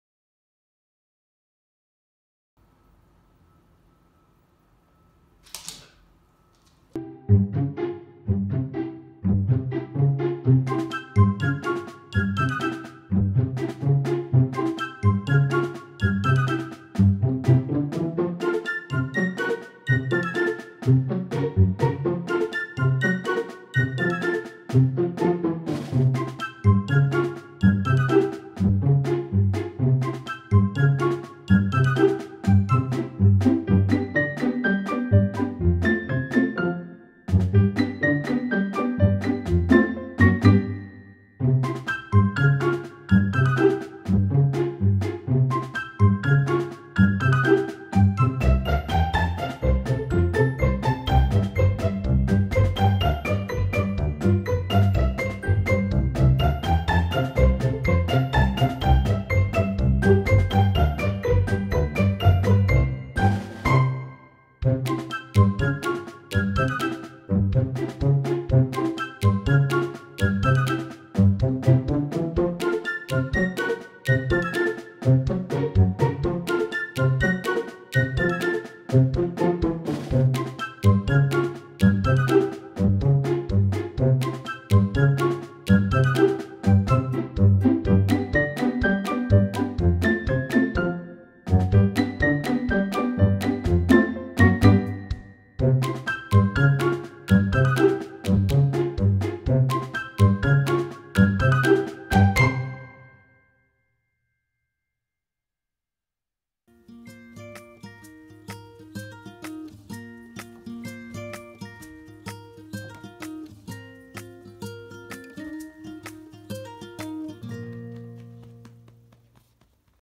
My Dog is Freaked Out by Hidden Cat
Although Cello was afraid of her, she was a very sweet kitty!
I fostered her from a local shelter, and now she got adopted to a happy family. 😊